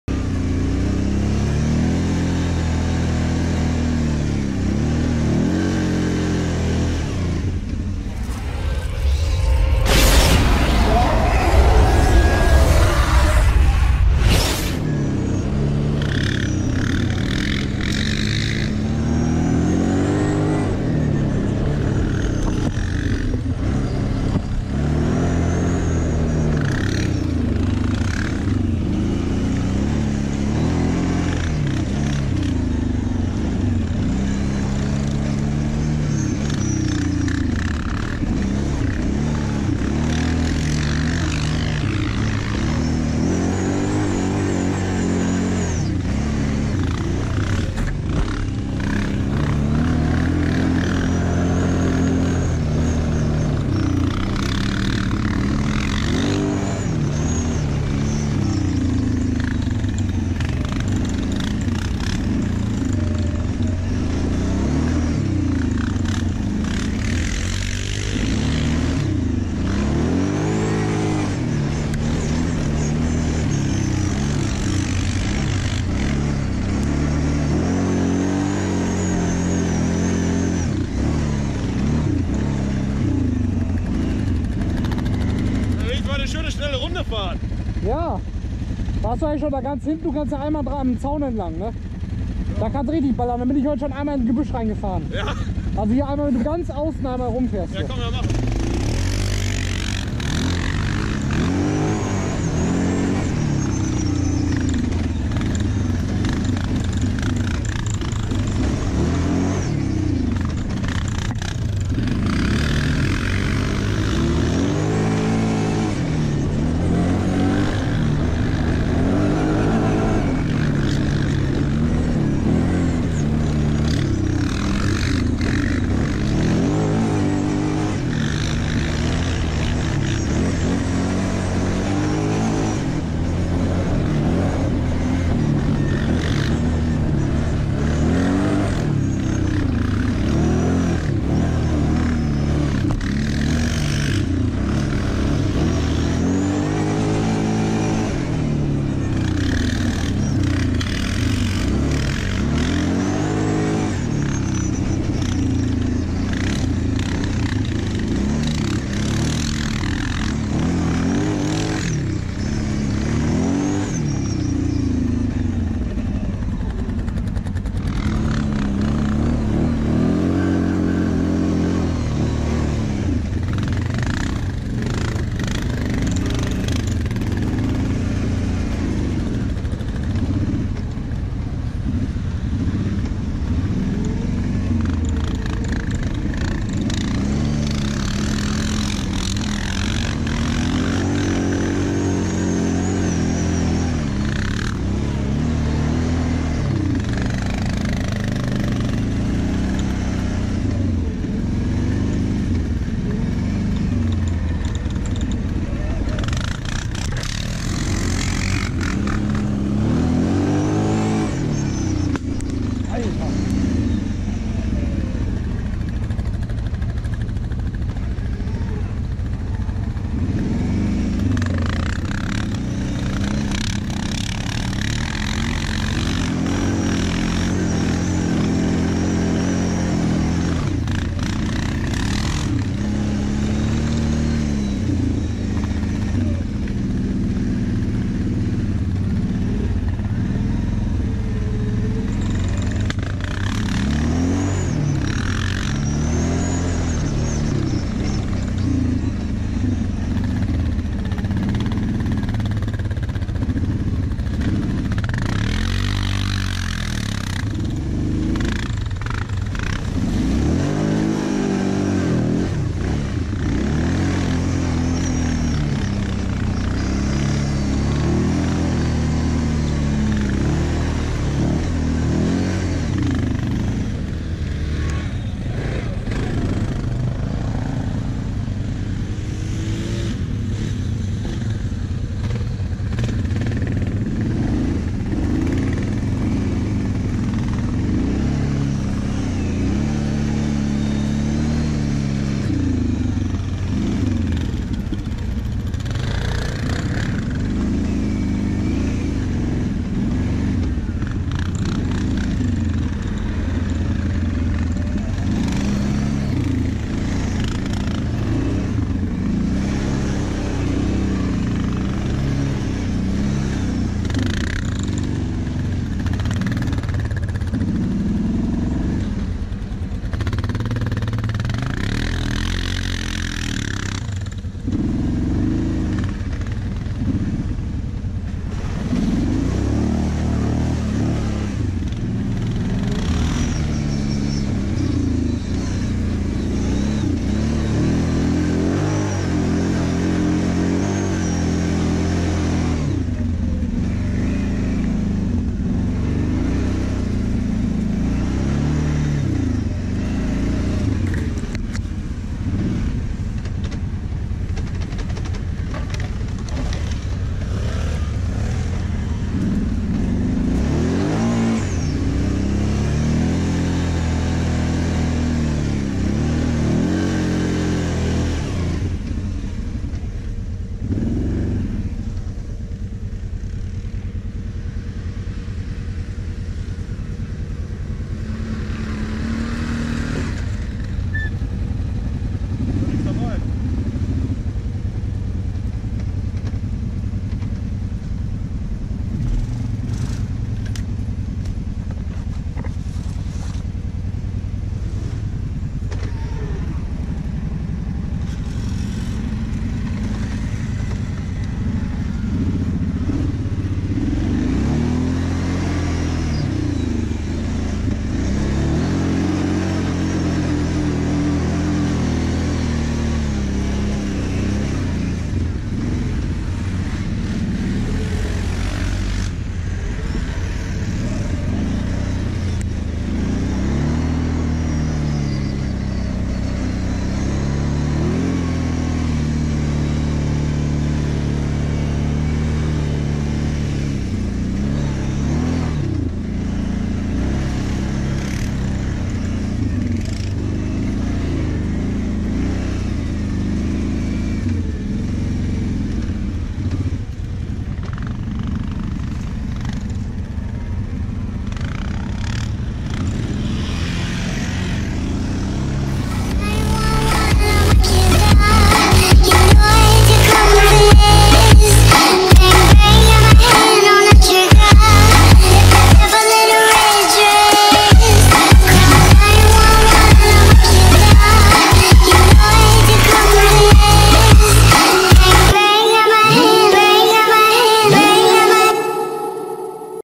Einfach Gas Geben | Kingquad und KVF | Funpark Meppen März 2020 | Quad Crew Münsterland | BI0M0RPH
Am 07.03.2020 waren wir mit 40 angemeldetetn Fahrzeugen von der "Quad Crew Münsterland", "Quad Buddy's ASD", "Quadferkel" und "Flitzpiepen" im Funpark Meppen.

Vehicles in the video:
-Kymco Maxxer 300 Wide
-Kymco Maxxer 450i 4x4
-Online X9.1

Follow me and my people too:
Instagram Biomorph: